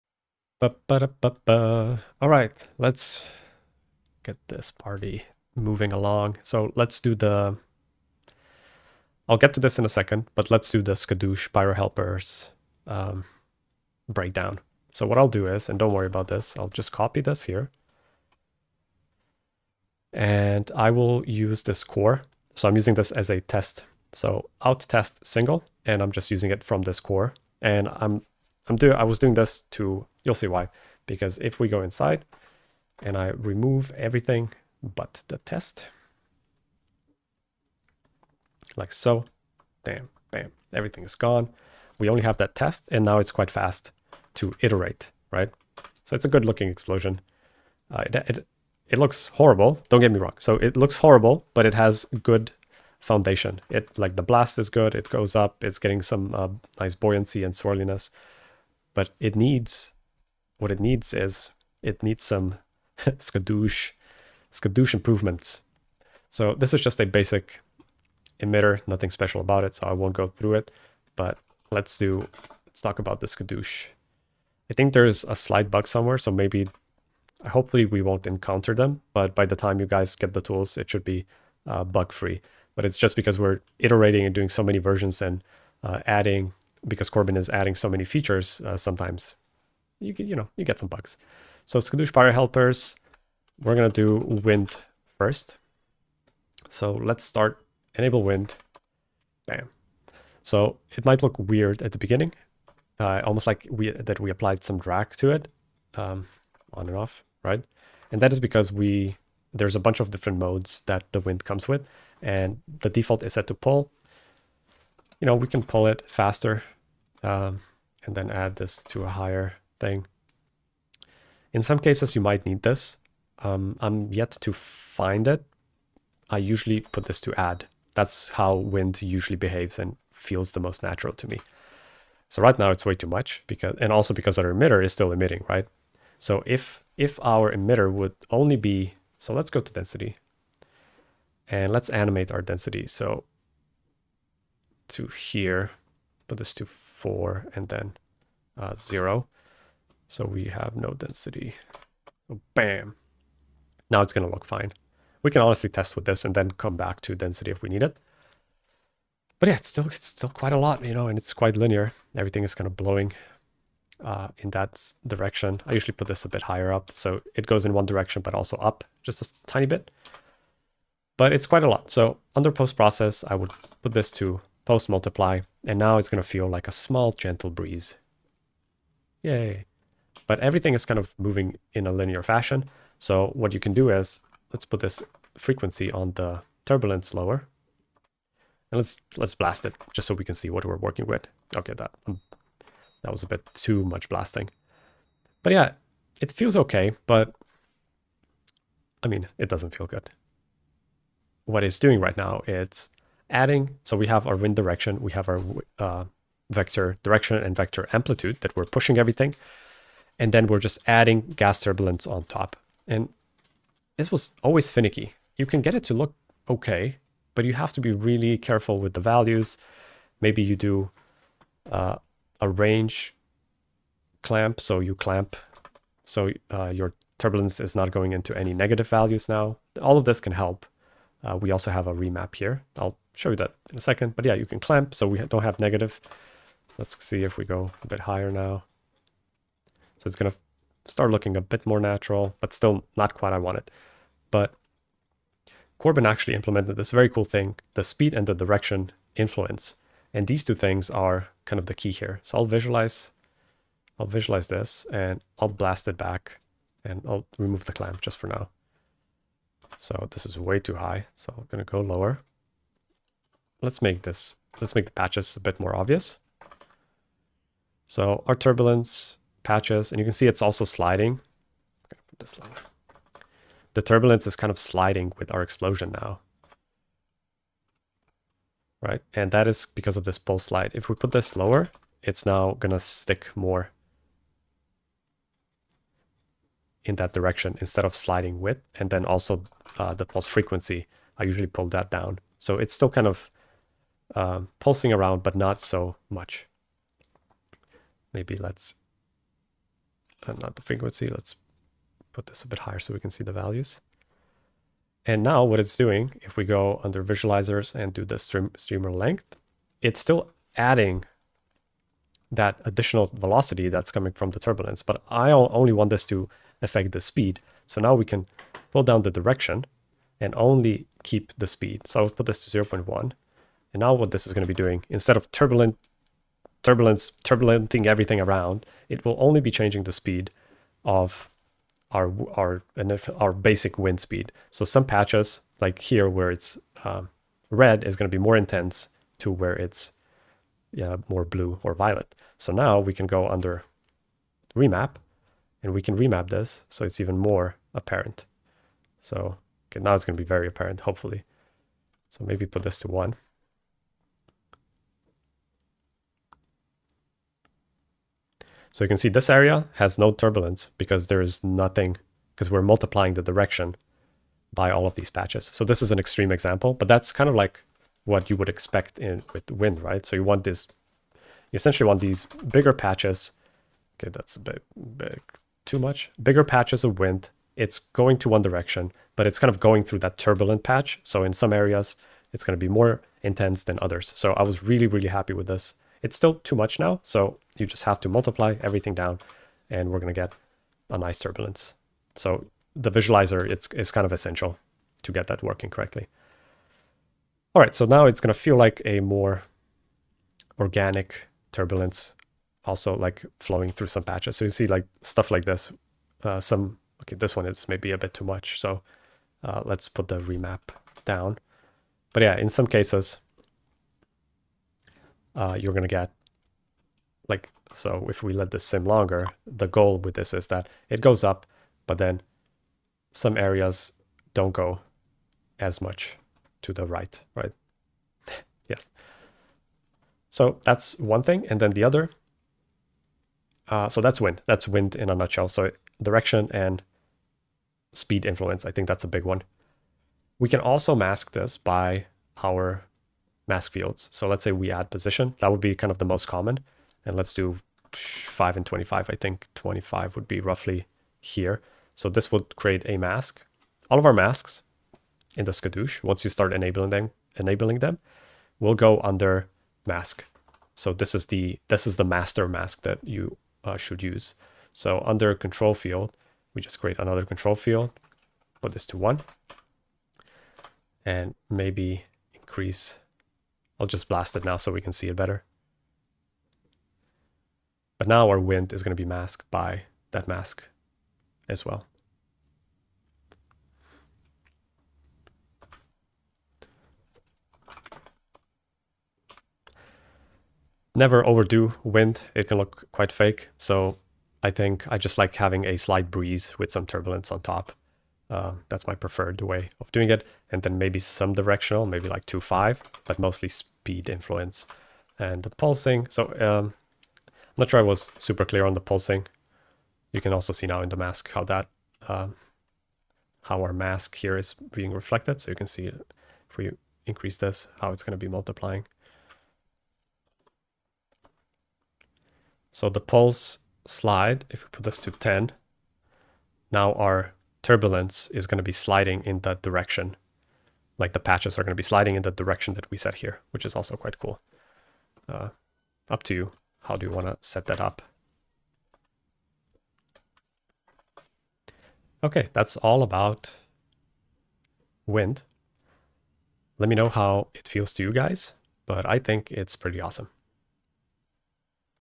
Skadoosh Pyro Helpers - How to Use - Episode 1
Get the Skadoosh Pyro Helpers for FREE!
Special thanks to SideFX for making that possible.

In this workshop, Urban Bradesko will explore the journey of creating explosions in Houdini. During the first few weeks, we will focus on art direction and building our emitters. Emitters constitute 80% of the appearance in explosions, and regardless of the solver used, they are essential for creating the look, shapes, and speed of the explosion. Without a solid emitter, you are merely testing your luck on how the explosion will be simulated, since the shapes change every time you alter the resolution.

We will also take a look at building the shaders.

The subsequent weeks will be dedicated to practical, shot-based projects like those seen in the teaser. We recreated famous explosion shots and reverse-engineered how they were done.

We will be tackling rendering and compositing as well.

These are just some of the shots we will be recreating. More will come when we release the full trailer soon.

The workshop content is made by Julen Elia, Ismael Martinez, Ayush Ramana & Urban Bradesko